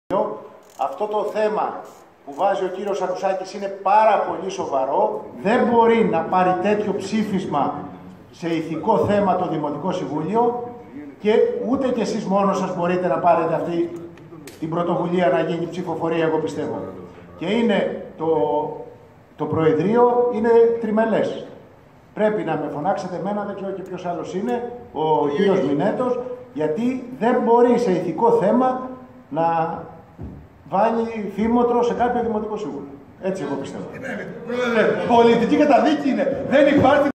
Σκλάβος: Δεν μπορεί να υπάρξει τέτοιο ψήφισμα το Δημοτικό Συμβούλιο
Συζήτηση για τον Πλατύ Γιαλό